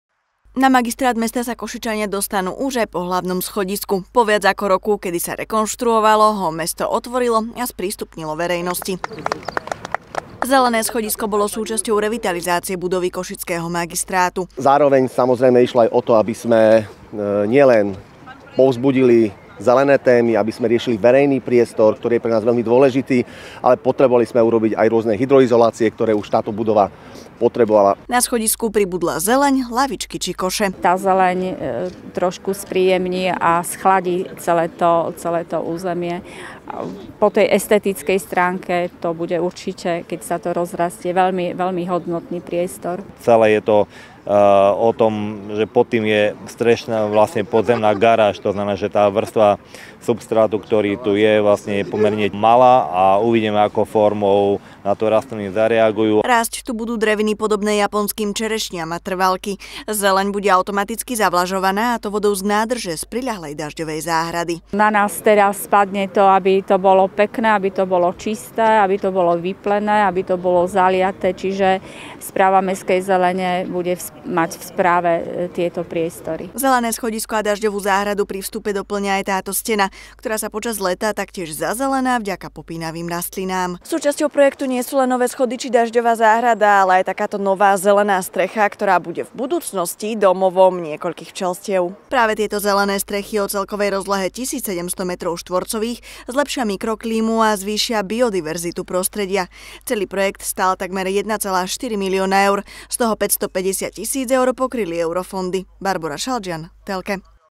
Magistrát má zelené strechy aj schody
Na magistrát mesta sa Košičania dostanú už aj po hlavnom schodisku. Po viac ako roku, kedy sa rekonštruovalo, ho mesto otvorilo a sprístupnilo verejnosti.

Zelené schodisko bolo súčasťou revitalizácie budovy košického magistrátu.

Na schodisku pribudla zeleň, lavičky či koše.

Rásť tu budú dreviny podobné japonským čerešniam a trvalky. Zeleň bude automaticky zavlažovaná vodou z nádrže z priľahlej dažďovej záhrady.

Zelené schodisko a dažďovú záhradu pri vstupe dopĺňa aj stena, ktorá sa počas leta taktiež zazelená vďaka popínavým rastlinám.

Zelené strechy o celkovej rozlohe 1 700 m2 zlepšia mikroklímu a zvýšia biodiverzitu prostredia. Celý projekt stál takmer 1,4 milióna eur, z toho 550 000 eur pokryli eurofondy.